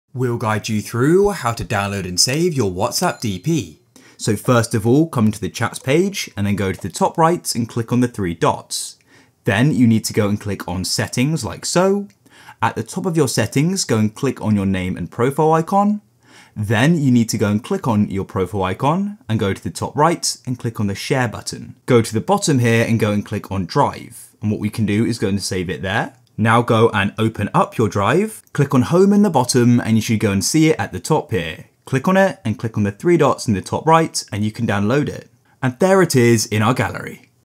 How To Save WhatsApp DP Picture - Full Guide
Learn how to save and download whatsapp dp picture in this video.

GuideRealm